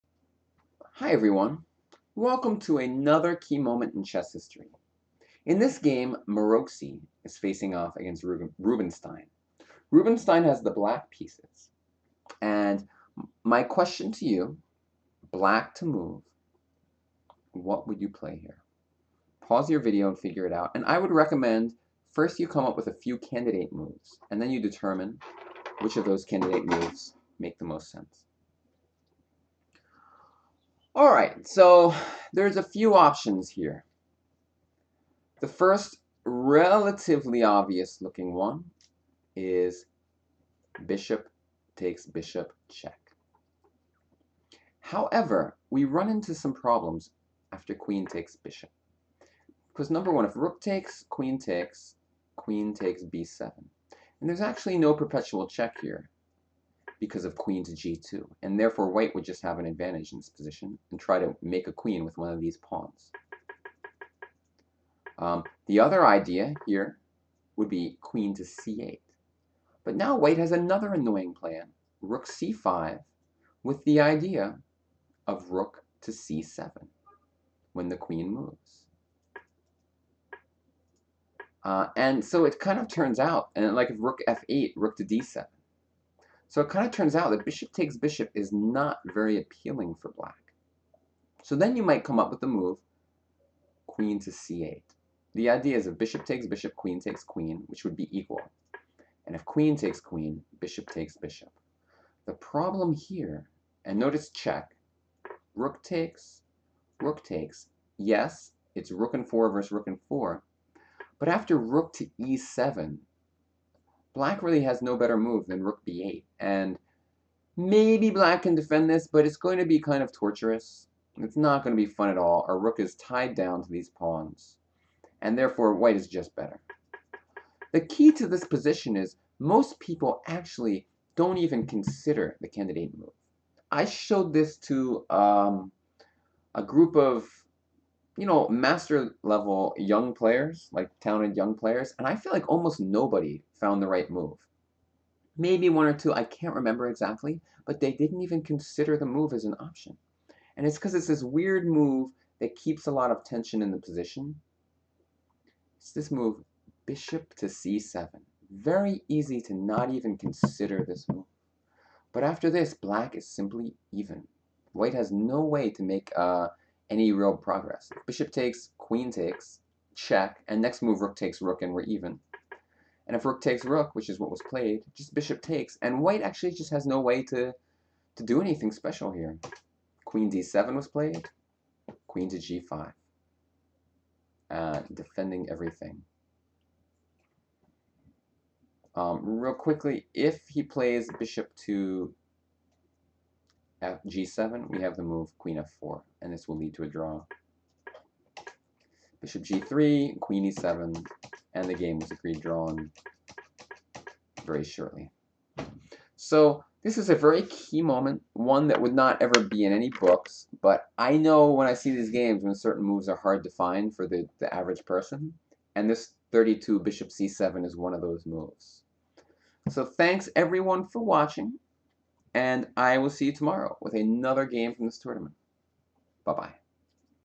Key Moments in Chess History #185: Maroczy v Rubinstein, Karlsbad 1907, Rd 17
Round 17 of the Karlsbad Tournament of 1907, won by Akiba Rubinstein.